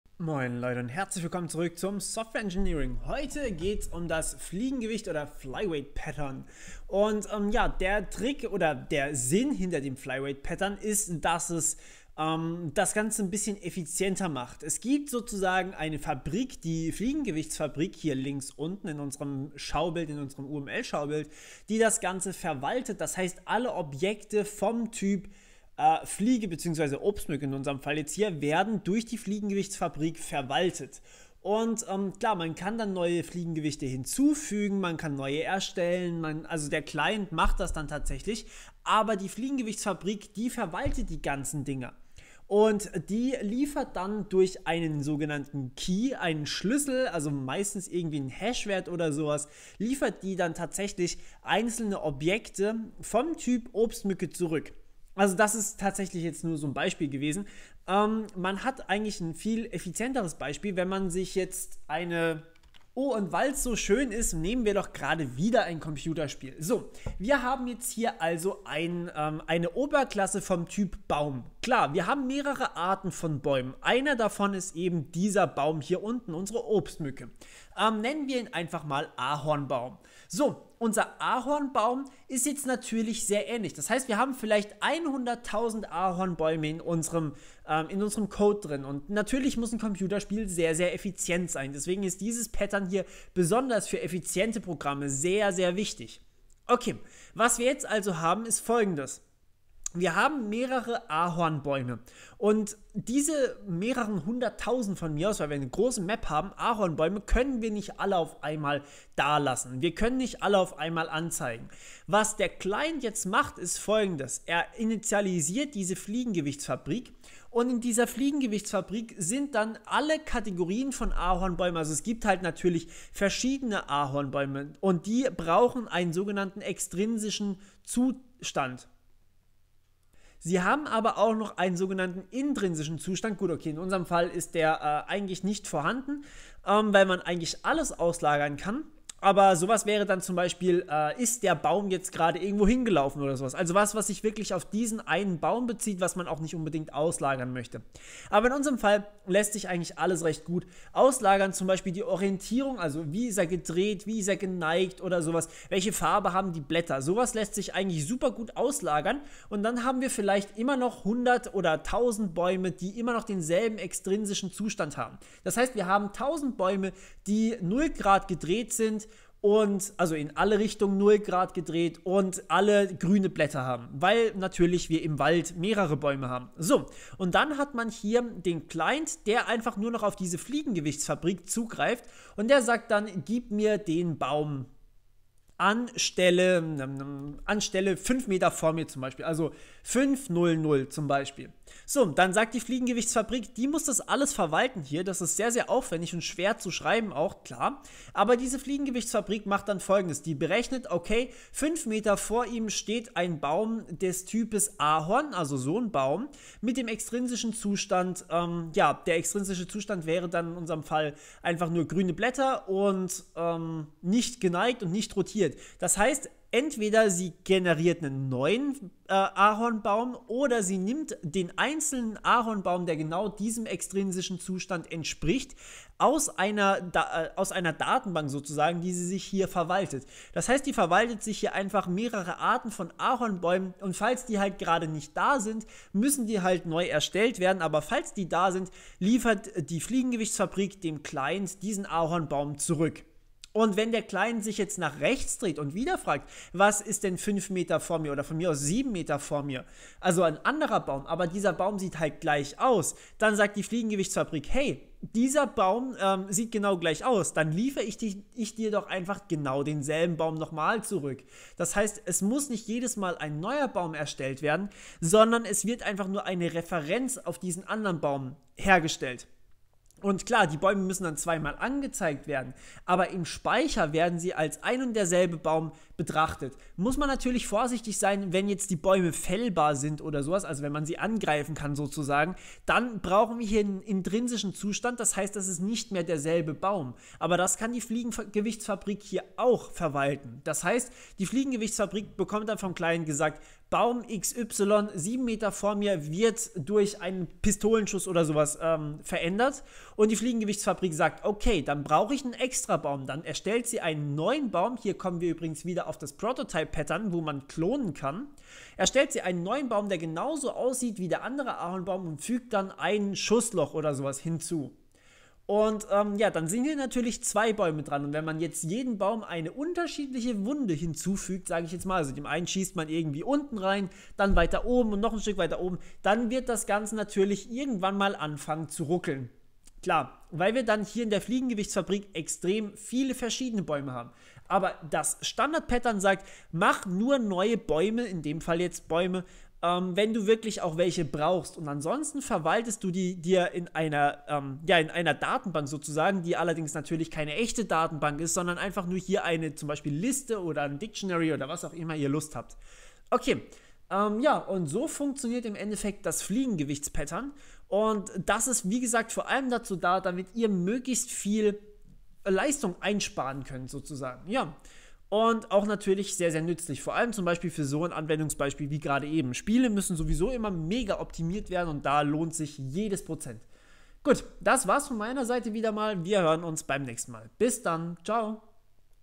Softwareengineering Tutorial #35 - Das Flyweight / Fliegengewicht Pattern
In diesem Tutorial schauen wir uns das Fliegengewichtspattern an.